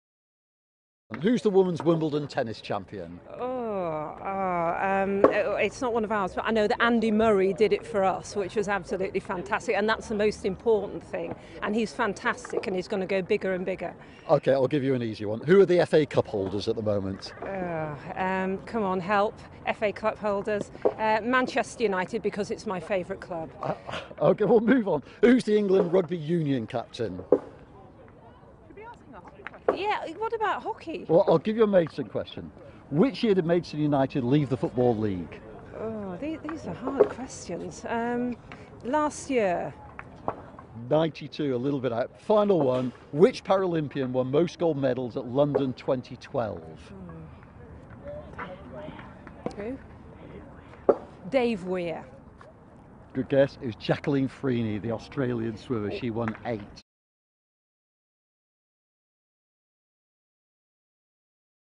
Cringe! Watch Sports Minister Helen Grant's embarrassing fail
A five-nil drubbing for Sports Minister: Quiz failure MP can't even name FA Cup winners